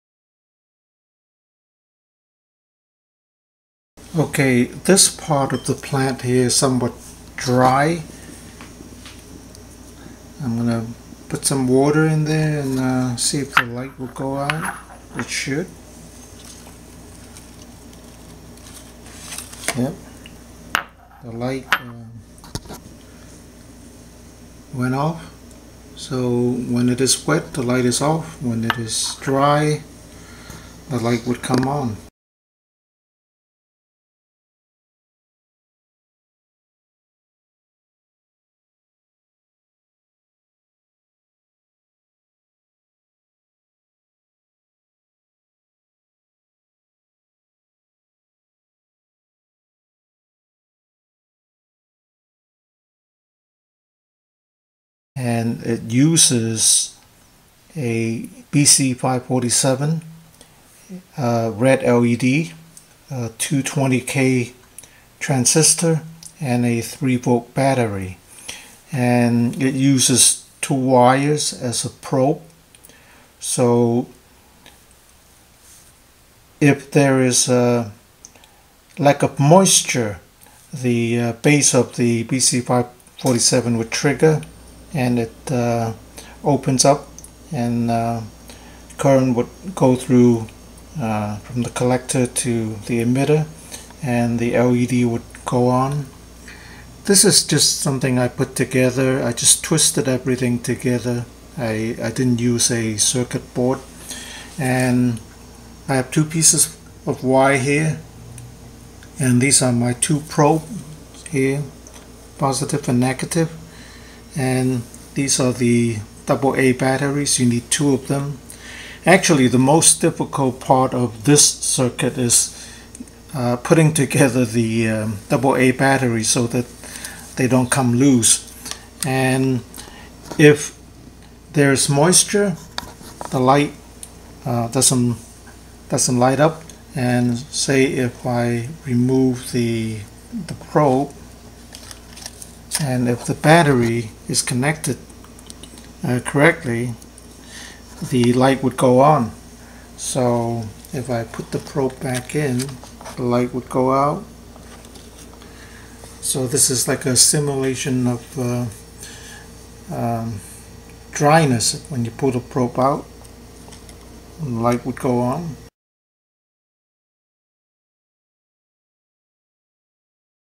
Soil dryness sensor with red LED
I was looking for a moisture sensor, instead I found this dryness sensor for plant soil. This dryness sensor is very simple and uses just a few components and a 3 volt power source.  By inserting 2 wire probes into your potted plant, the sensing circuit will switch on the red LED when the soil is dry. Upon watering or when it senses moisture, the LED would go out.